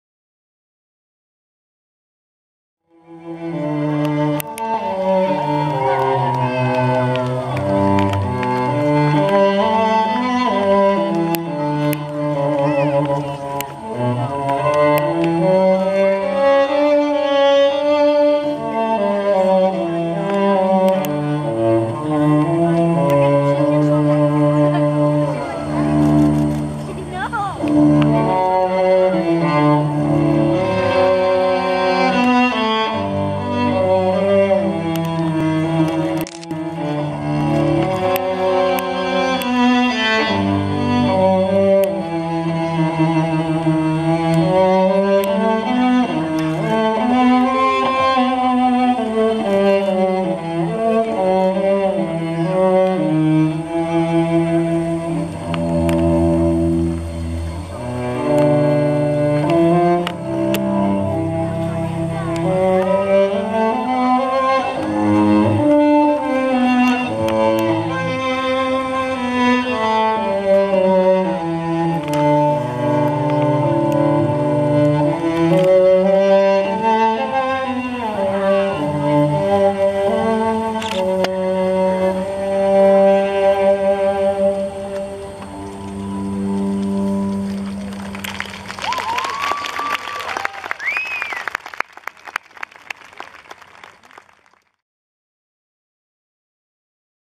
The Founder's Dance in Daylight - Amelia Rudolph - Founder/Artistic Director
Bandaloop came to Redding to perform at the 10th Anniversary of the Sundial Bridge in Redding, California. All of the were , but my favorite was this graceful, amazingly elegant dance by the Amelia Rudolph. Thank you Bandaloop for coming to our city of Redding!!